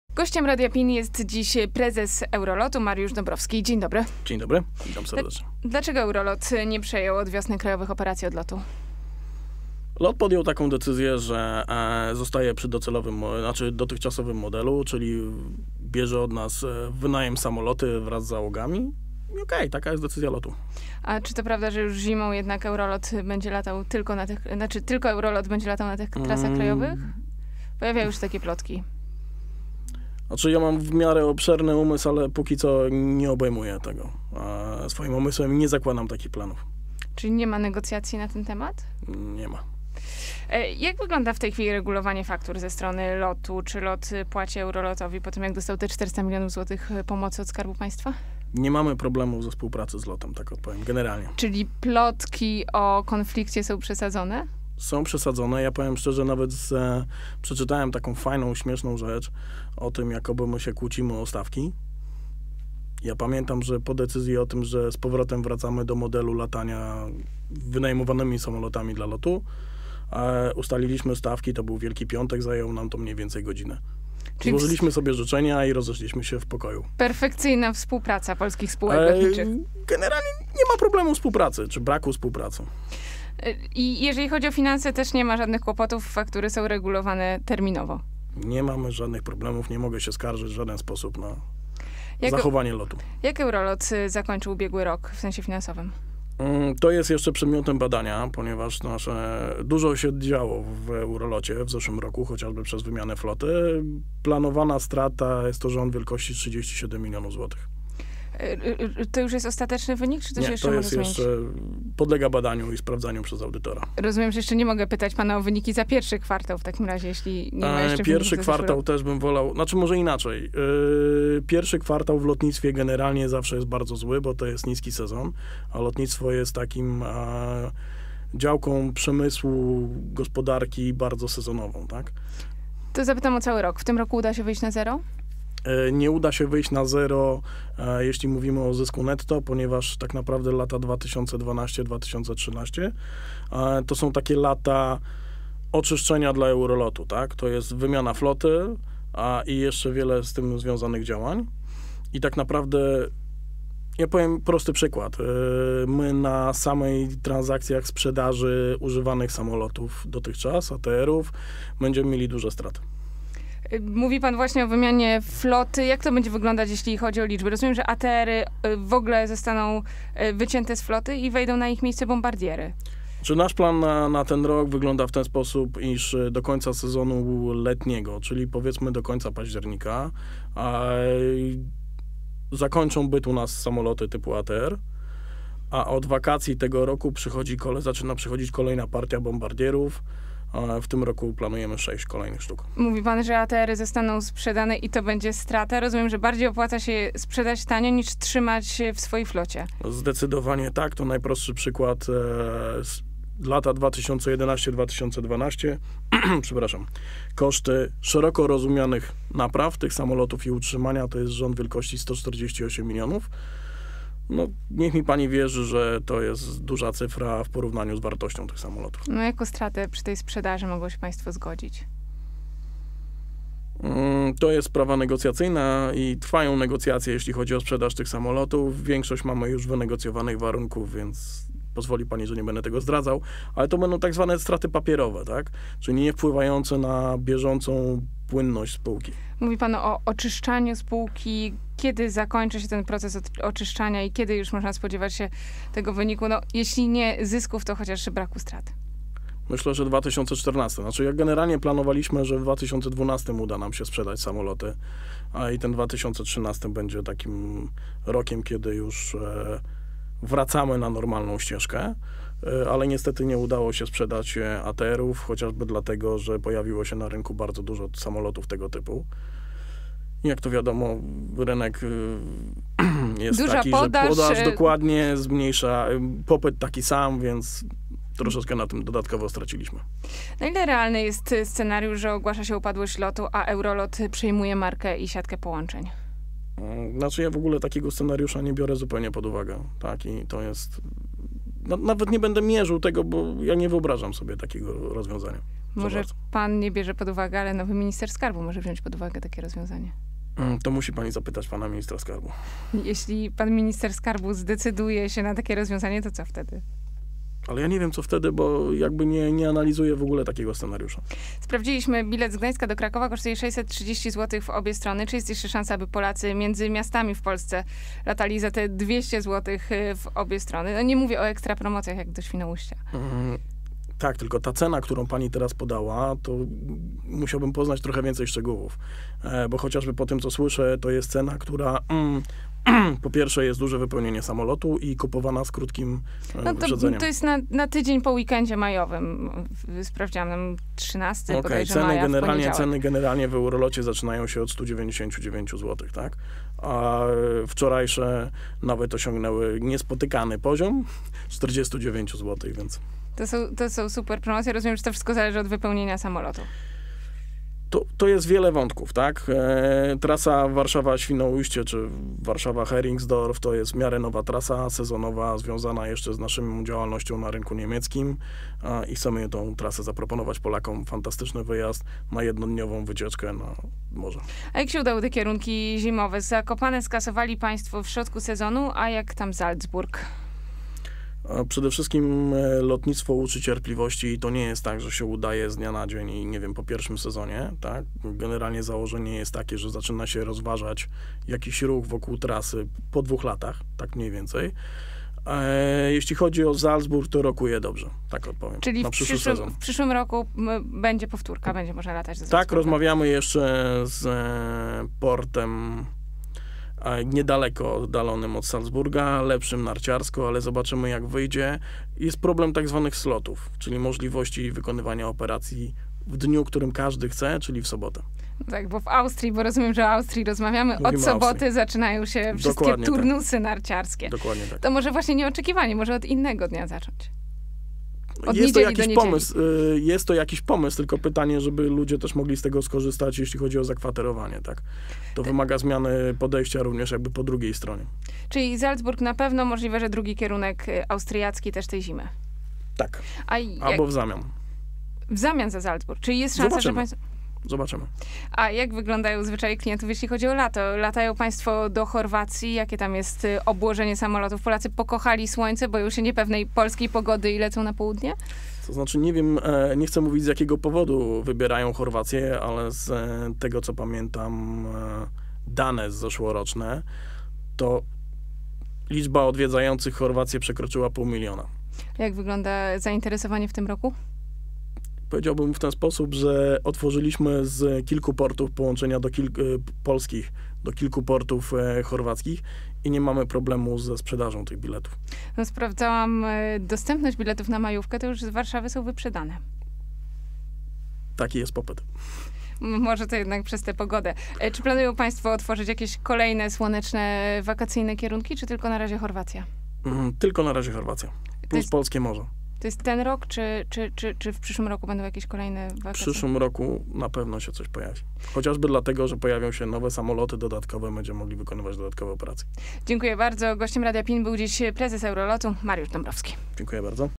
Mariusz Dąbrowski - gość Radia PiN
Gościem Agnieszki Witkowicz jest prezes Eurolotu, Mariusz Dąbrowski.